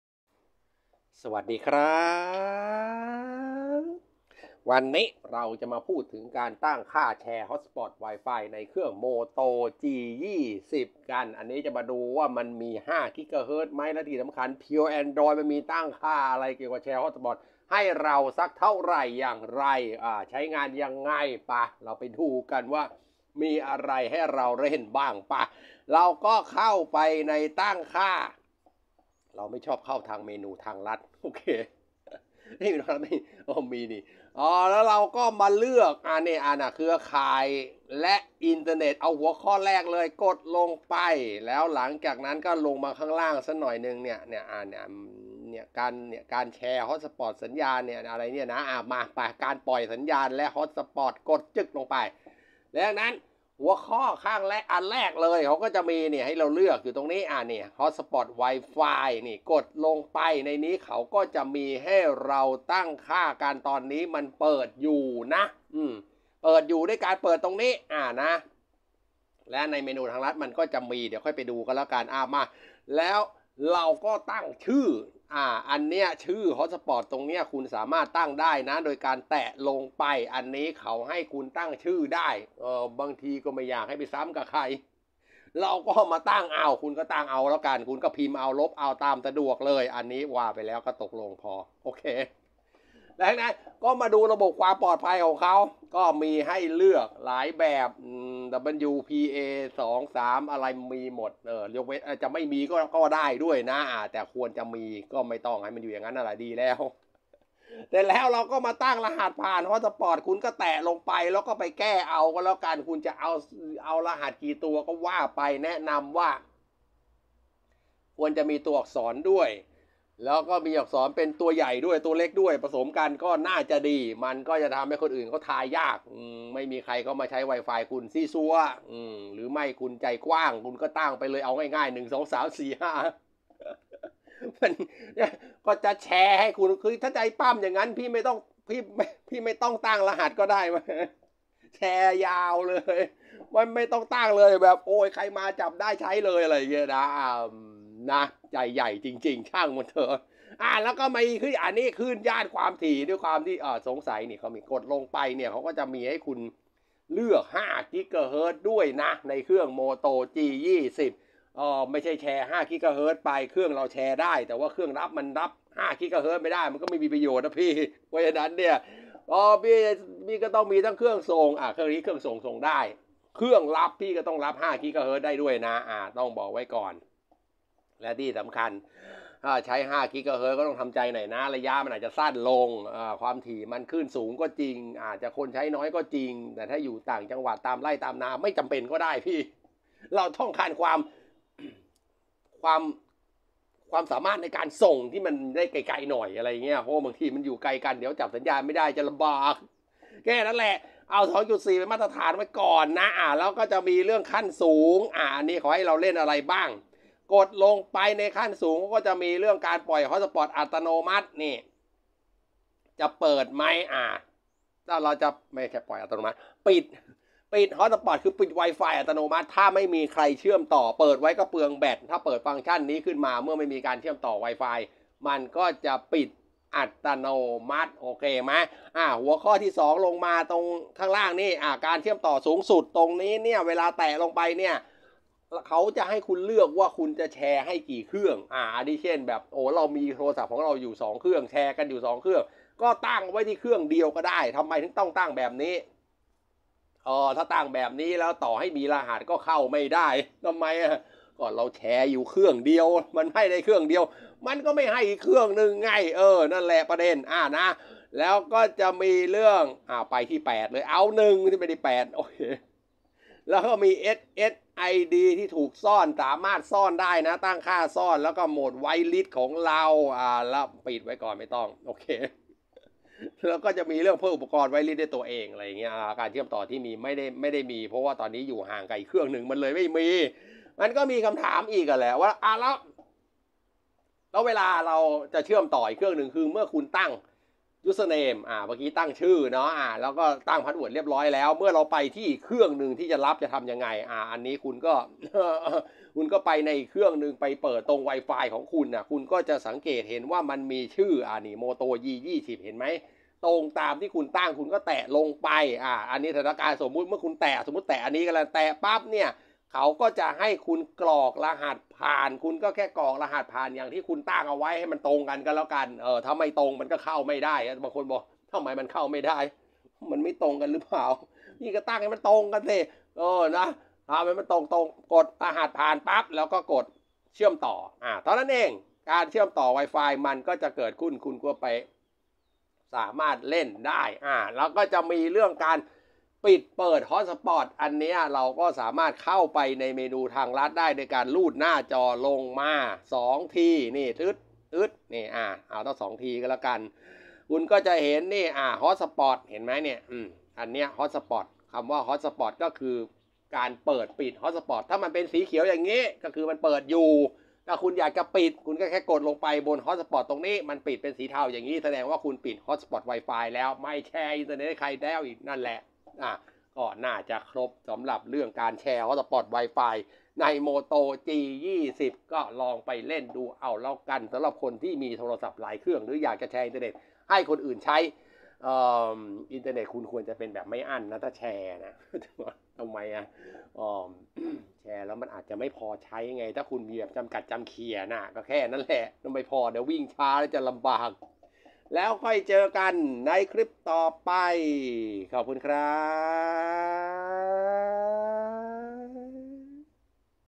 แชร์ hotspot WiFi ใน Moto G20
เรามาตั้งค่าแชร์อินเทอร์เน็ต เปิดใช้งาน hotspot WiFi ในเครื่องโทรศัพท์มือถือสมาร์ทโฟนโมโตโรล่า Moto G20 ซึ่งเป็น pure android 11 ทำอะไรได้บ้าง ดูว่า
มี WiFi 5 ghz ไหม
จำกัดการแชร์อินเตอร์เน็ตได้ไหม
ตั้งค่าเปลี่ยน username password ทำไง
และสามารถตั้งค่าอะไรได้บ้าง